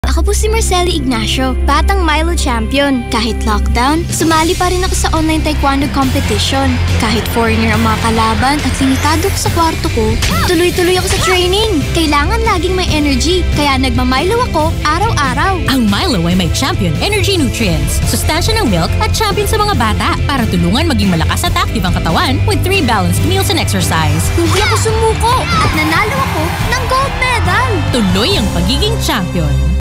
Milo Radio Commercial 2021 30s
Mula noon hanggang ngayon, tuloy ang kanyang pangarap maging champion with MILO!

Milo. Ituloy ang pagiging champion. Mag-Milo Everyday!

DRPS Two Episode No. 38: Milo Radio Commercial 2021 30s